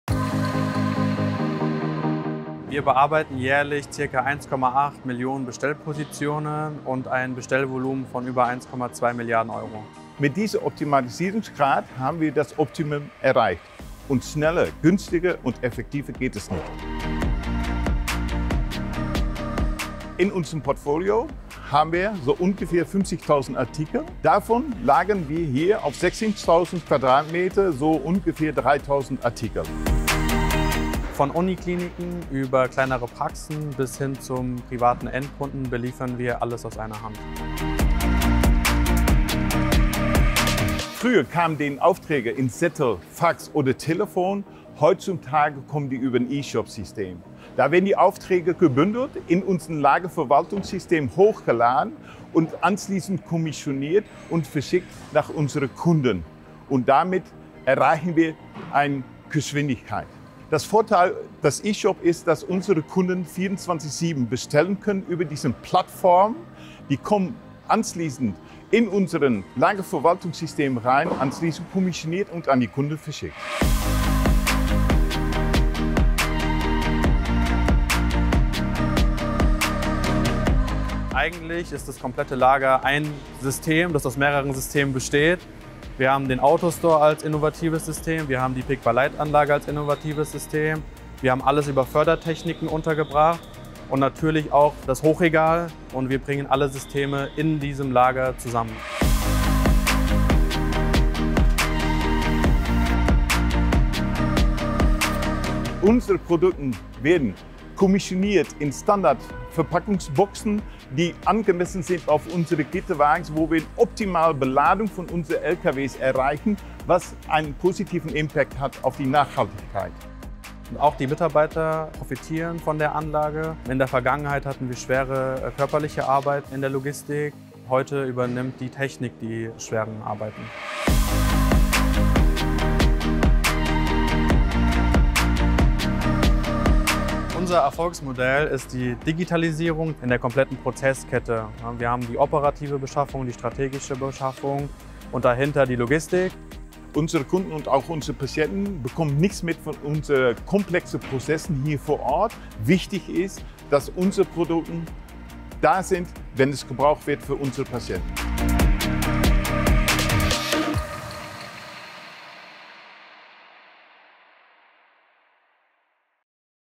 Asklepios Zentrallager - Versorgung von Kliniken und Gesundheitseinrichtungen
Bundesweit größtes und modernstes Logistikzentrum für die Versorgung von Kliniken und Gesundheitseinrichtungen. Die Asklepios Kliniken Gruppe steuert die Logistik für ihre bundesweit rund 170 medizinischen Einrichtungen vom neu errichteten Zentrallager in Bad Oldesloe (Schleswig-Holstein) aus.
Das vollautomatisierte Hochregallager verfügt über einen Autostore mit Robotern und bietet 21.000 Palettenstellplätze auf 16.000 qm Hallenfläche. Gelagert und kommissioniert werden rund 3.000 unterschiedliche Produkte – von Spritzen und Kanülen über FFP2-Masken und Schutzkittel bis zu Verbandsmaterial. Gearbeitet und ausgeliefert wird rund um die Uhr.